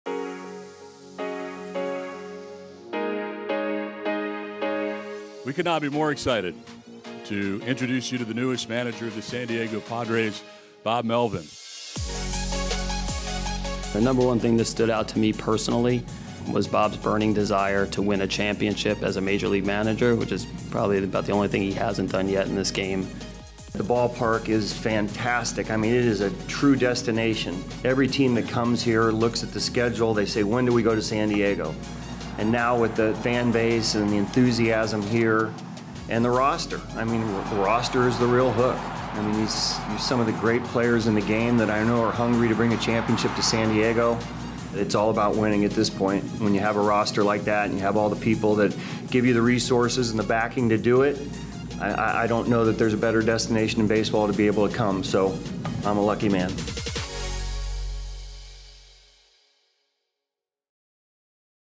Bob Melvin's first day as Padres manager
Welcome to San Diego, Bob Melvin! The 22nd manager in franchise history is ready to go to work.